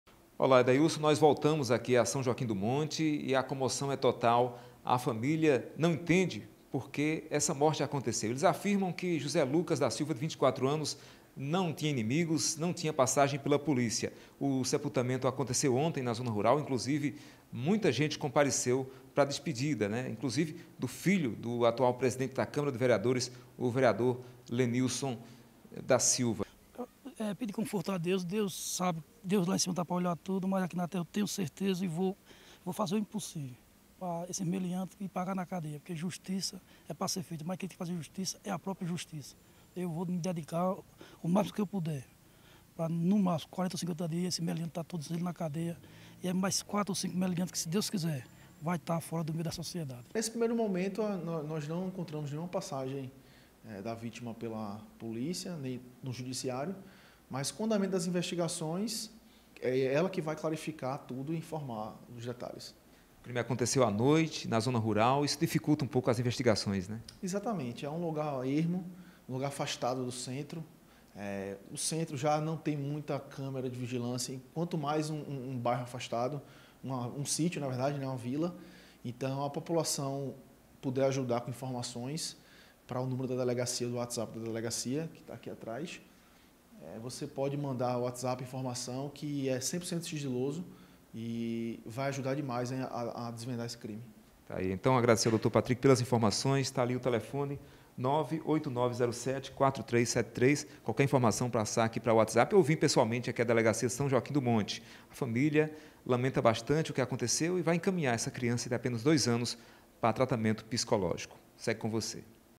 Polícia pede ajuda da população para encontrar suspeitos da morte de filho de vereador
A Delegacia de São Joaquim do Monte, no Agreste de Pernambuco, está investigando o homicídio cometido contra José Lucas da Silva, 24 anos, filho do presidente da Câmara de Vereadores do município.